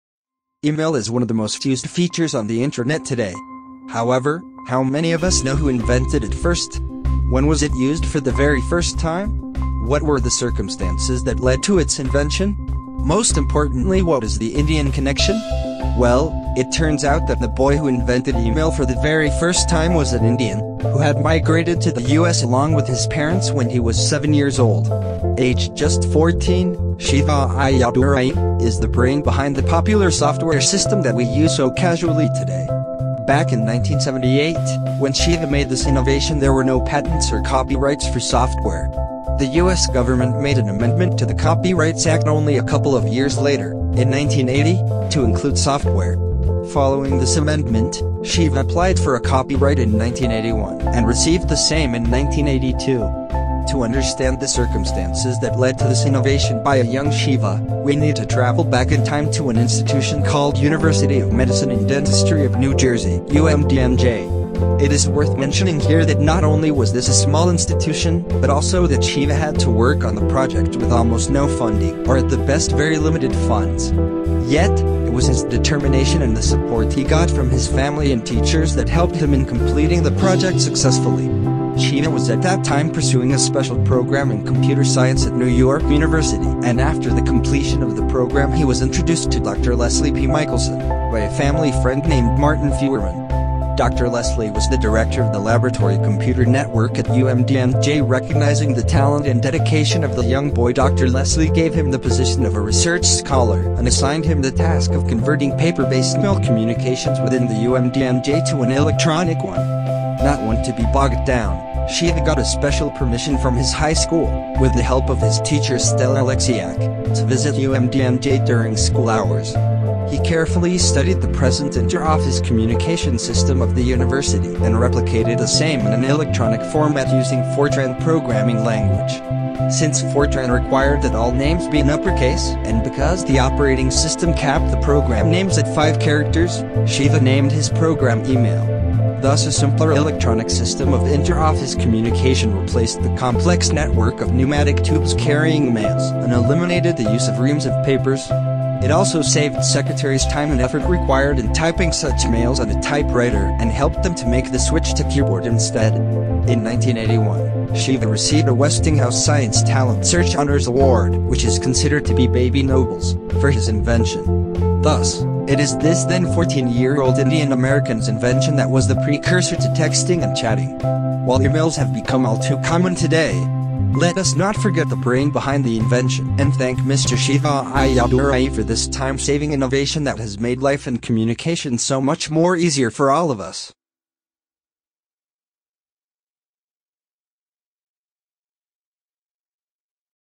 Who Invented Email, Do You Know That ?
Email is one of the most used features on the internet today. However, how many of us know who invented it first? When was it used for the very first time? What were the circumstances that led to its invention? Most importantly what is the Indian connection?

Well, it turns out that the boy who invented Email for the very first time was an Indian, who had migrated to the US along with his parents when he was 7 years old. Aged just 14